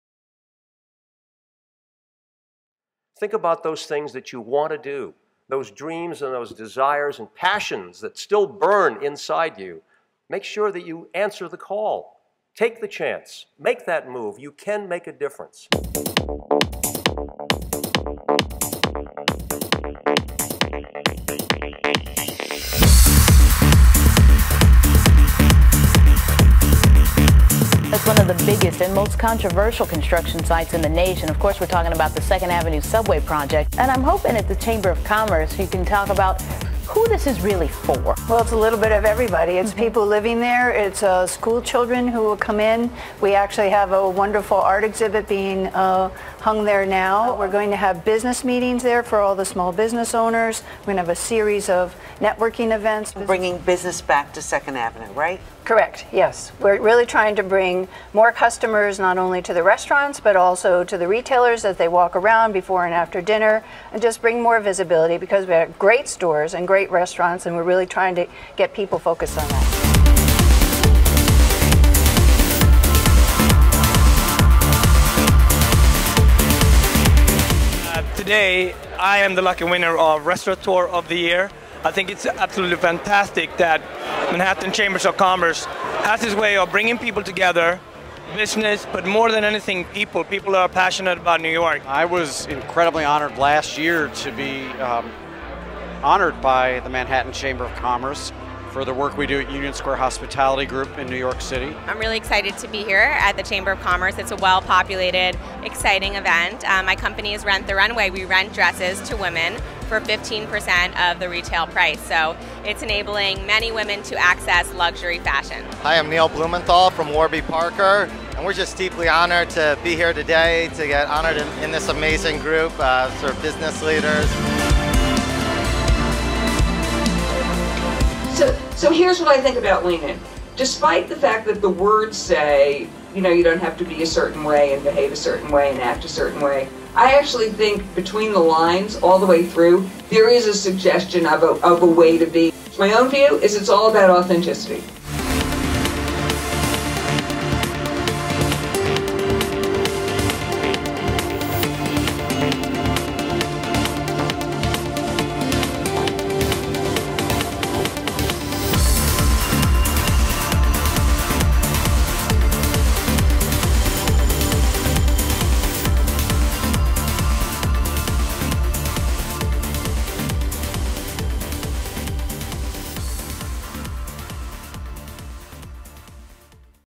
About the Manhattan Chamber of Commerce 2015
This video is about why you should join, sponsor and/or get involved with the Manhattan Chamber of Commerce. It includes Ernie Anastos (Fox5News), Warby Parker, Rent the Runway, Shelley Lazarus, Nancy Ploeger, Danny Meyer, and many more.